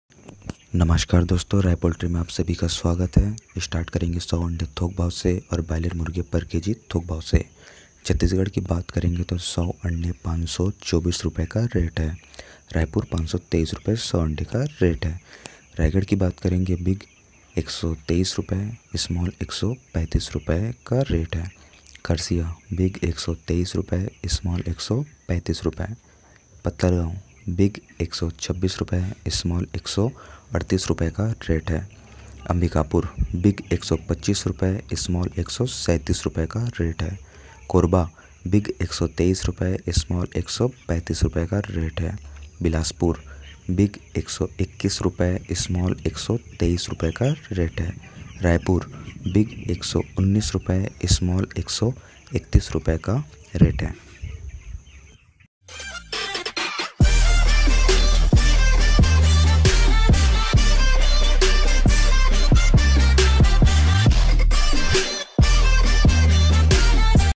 Today's Boilar and egg rate 10/10/2020
Chhattisgarh 100 अंडे 524 रुपए का रेट है।
रायपुर 100 अंडे 523 रुपए का रेट है
                     B        S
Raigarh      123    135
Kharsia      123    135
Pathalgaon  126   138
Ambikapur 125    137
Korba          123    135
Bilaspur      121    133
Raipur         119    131